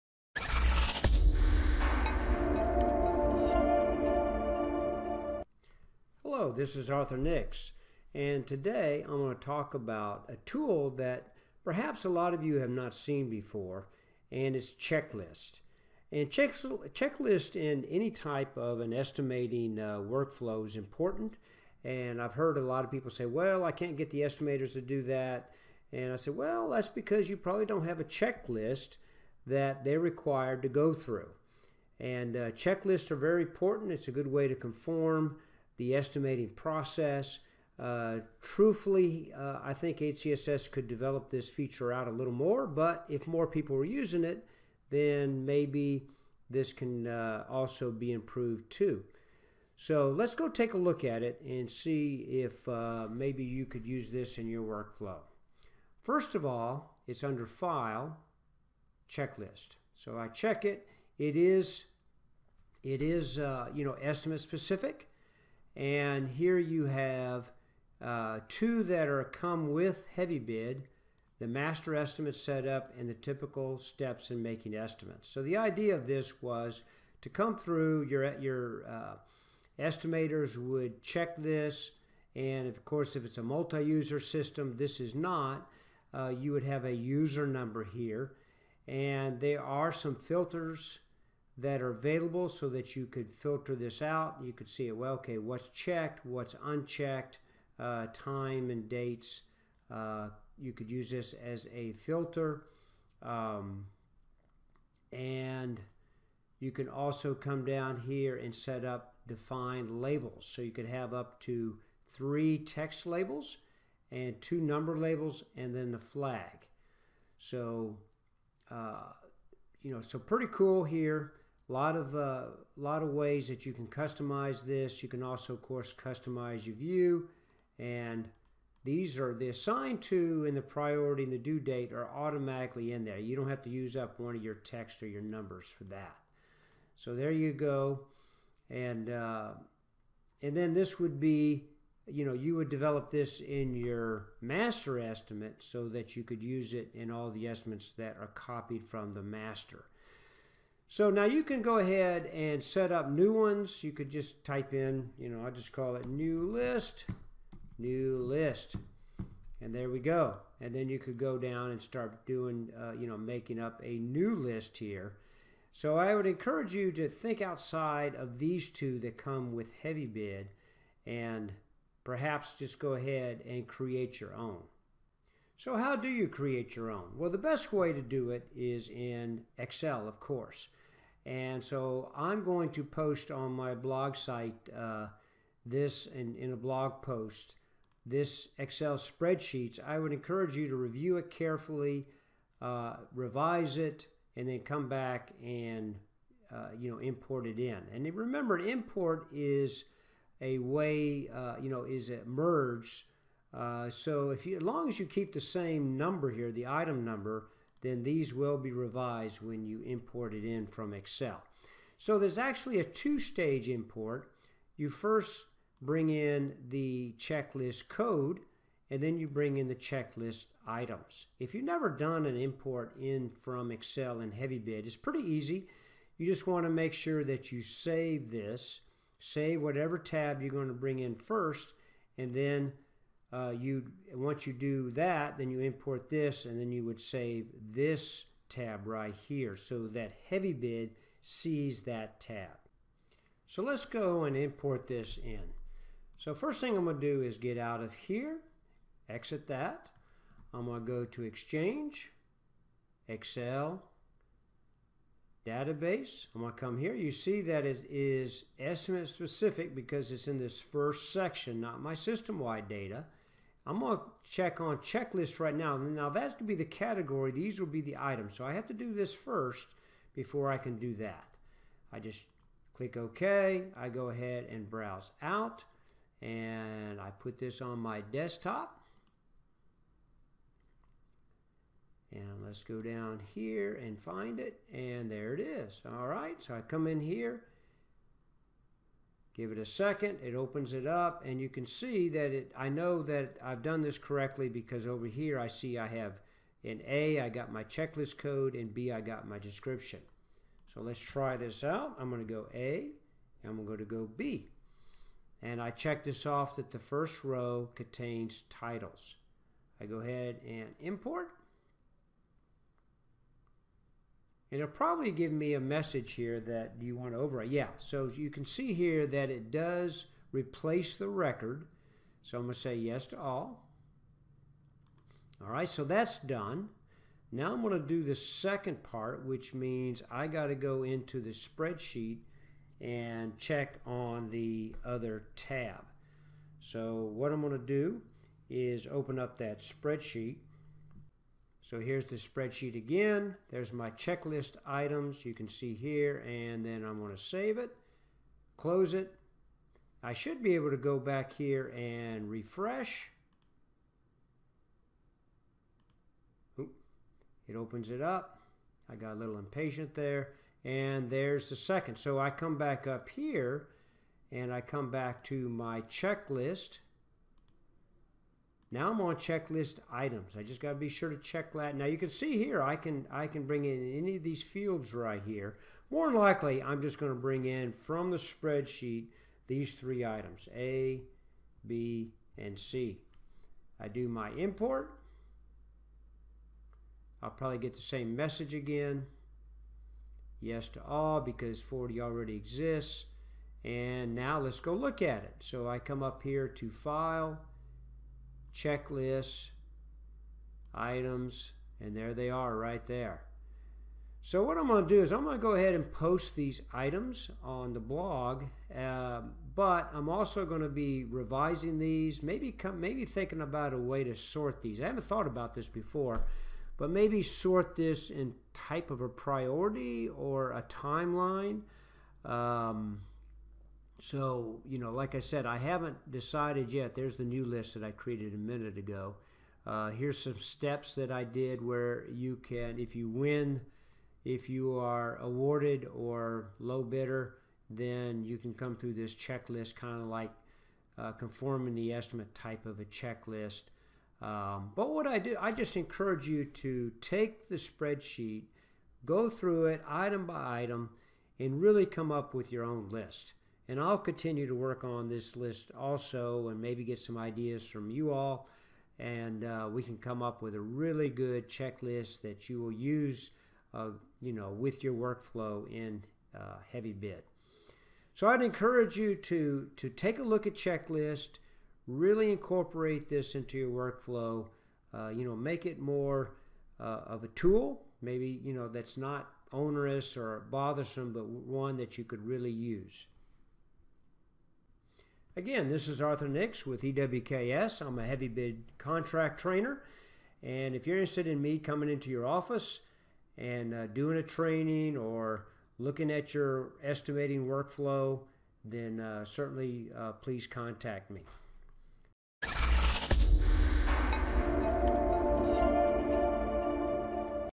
HCSS HeavyBid: Creating a Checklist
Checklists are a good way to keep different checklists running for the estimating team for an estimate. Like anything else in HeavyBid to really make use of this tool, you need to add and customize the checklists beyond what comes with the original HeavyBid install.
Here is a link to the original blog post where you can download my own personal checklist.